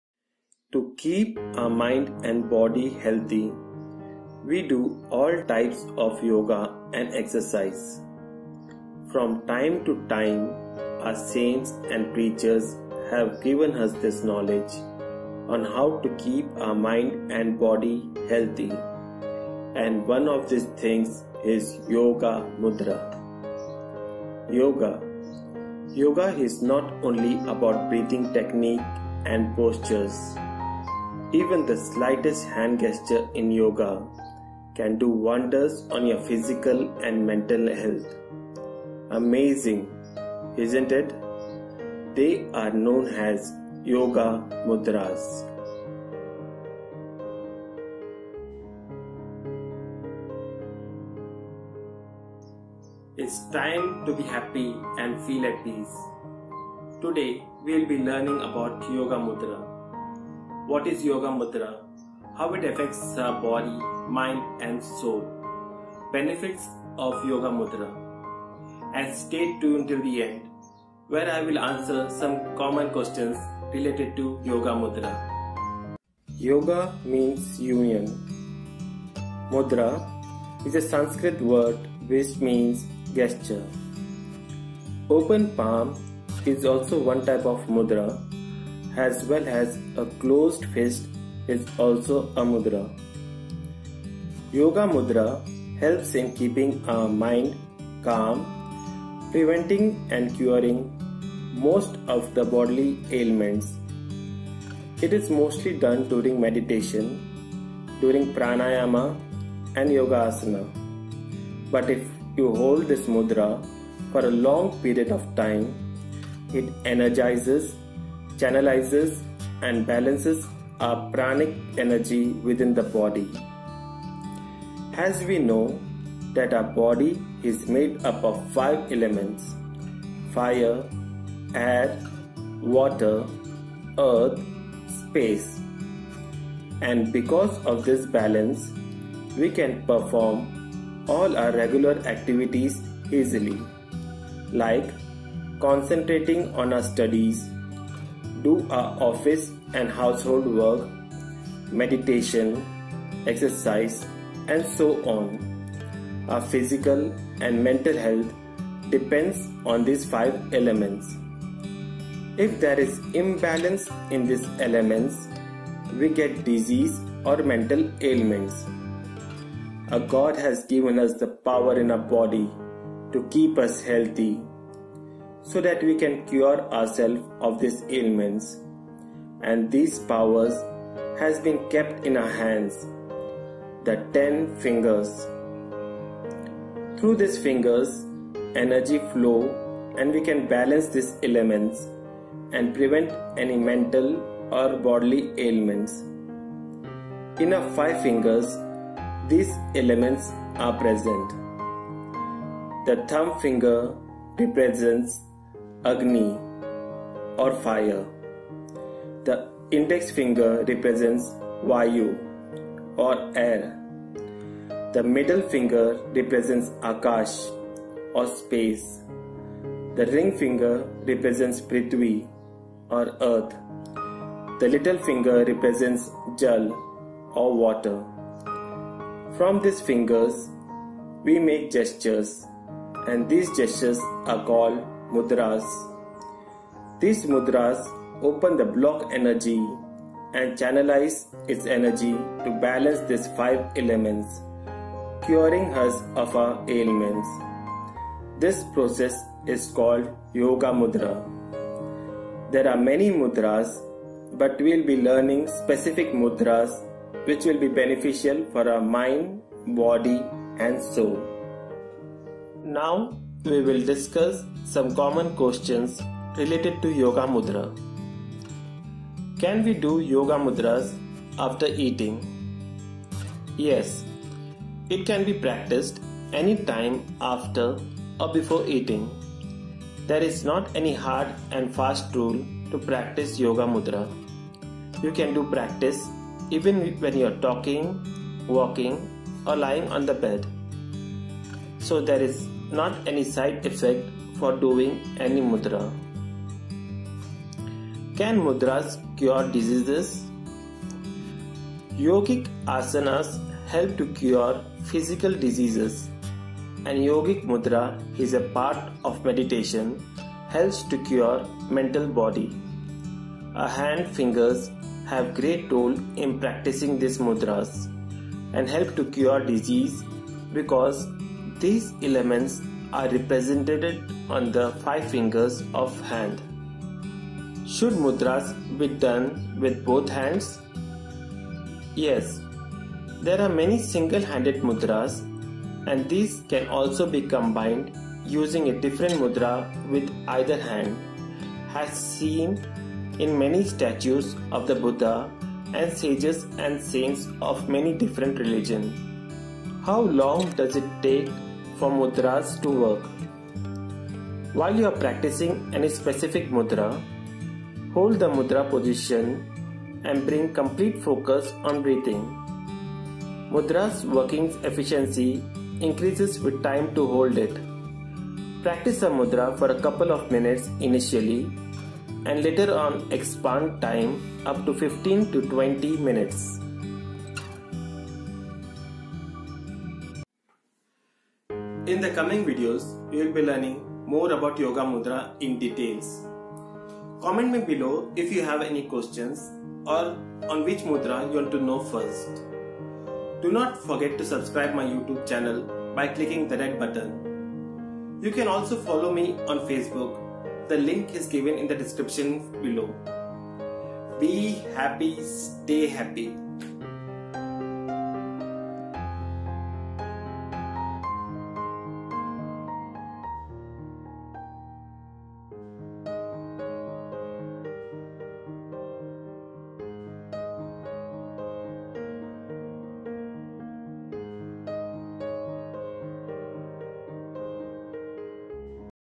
WHAT IS YOGA MUDRA? IMPORTANCE OF YOGA MUDRA!
YOGA MUDRA helps in keeping our mind calm, preventing and curing most of the bodily ailments. Through these fingers energy flows and we can balance these elements and prevent any mental or bodily ailments. In our five fingers these elements are present. From these fingers we make gestures and these gestures are called mudras. These mudras open the block energy and channelize its energy to balance these five elements curing us of our ailments. This process is called YOGA MUDRA.